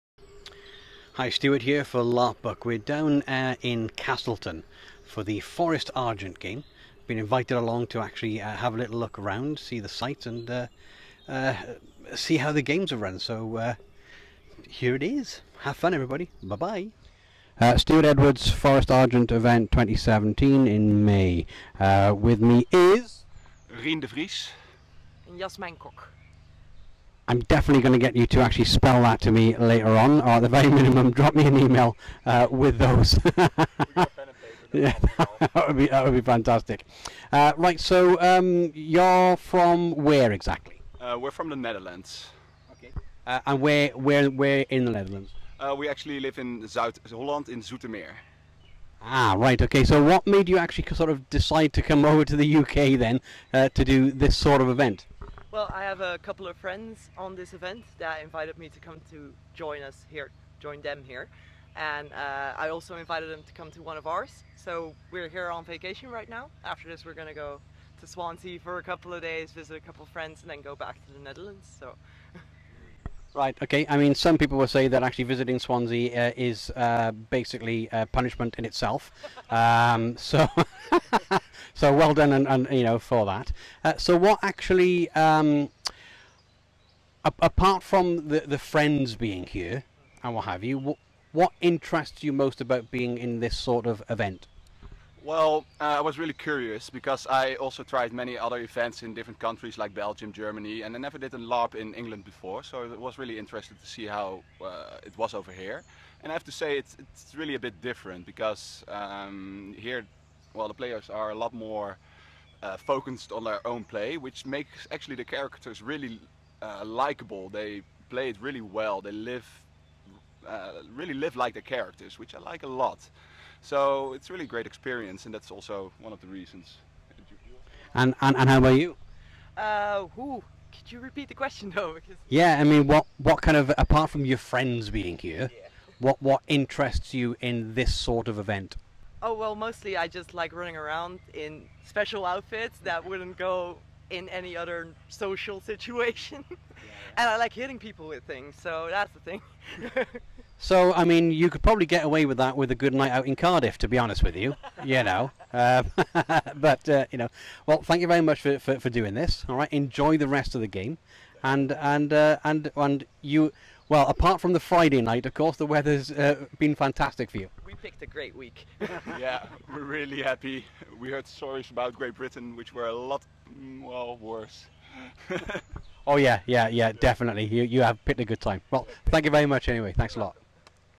Forest Argent Interview 2017
Forest Argent Convergence this May had a couple of International Larpers attending. We took a few minutes talk to them about visiting the UK to play in a LARP